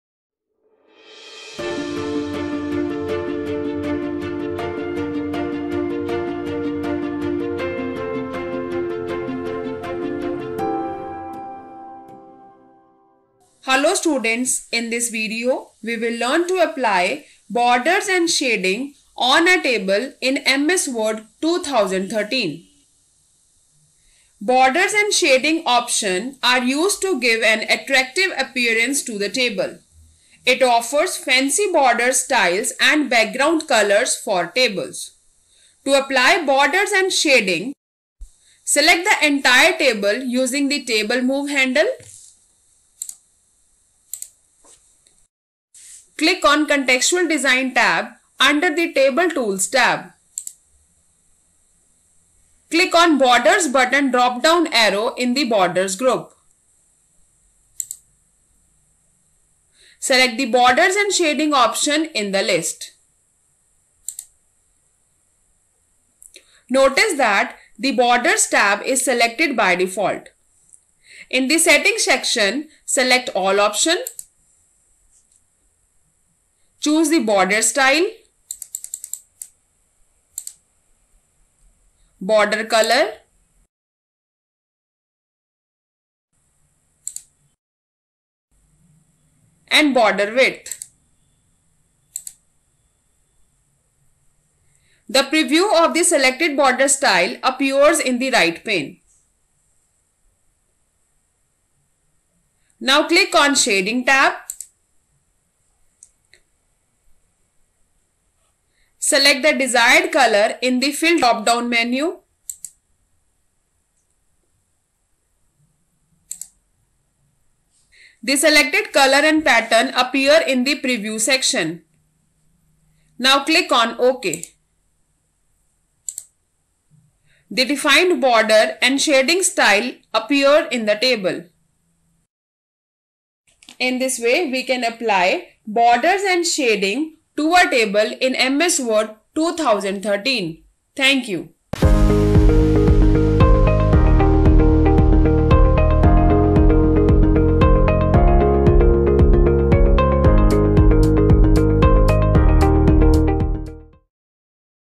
How to do Borders and Shading in MS Word 2013 in Windows 10
Borders and Shading in MS Word 2013 (Step by Step Learning)
Learn about Borders and Shading in MS Word 2013 in Windows 10
Class/ Grade - 5